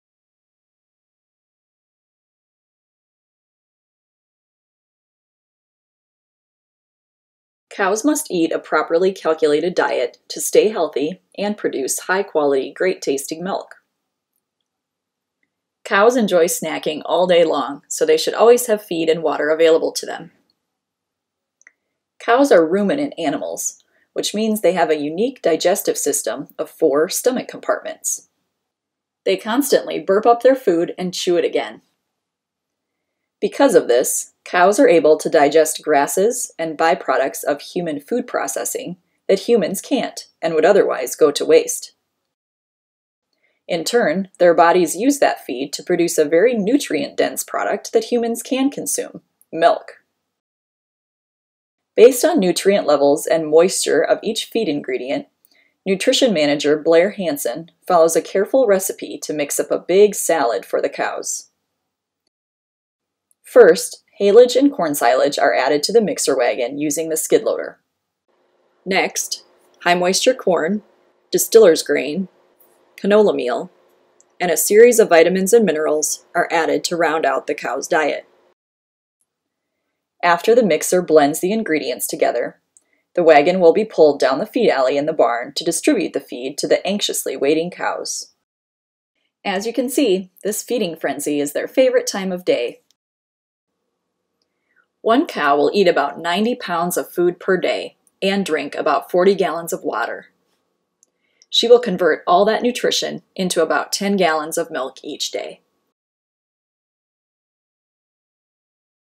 Hansen's Dairy Series 7: Feeding Cows
Cows must eat a properly calculated diet to stay healthy and produce high quality, great tasting milk. See what Holstein cows eat and how their "salad" is prepared every day at Hansen's Dairy in Hudson, Iowa.